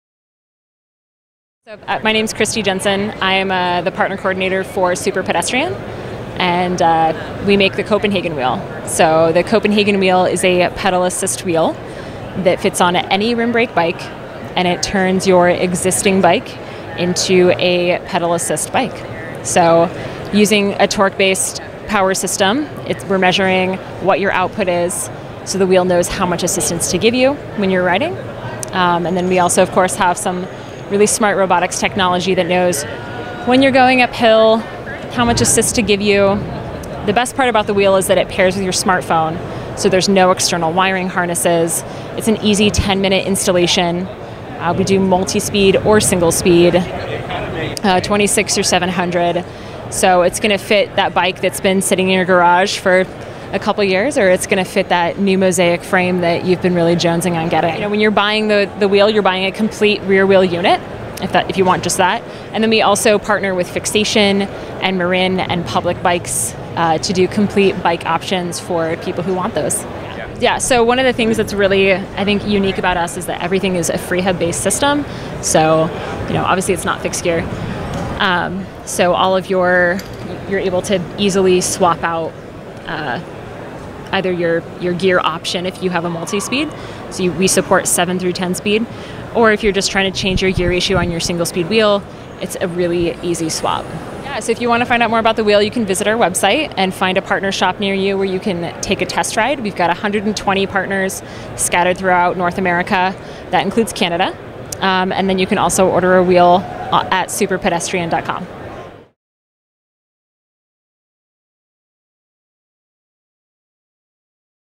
Copenhagen Wheel All-In-One Electric Bike System | Electric Bike Report
Christy Jensen from Superpedestrian gives us the overview of the Copenhagen Wheel all-in-one electric bike system.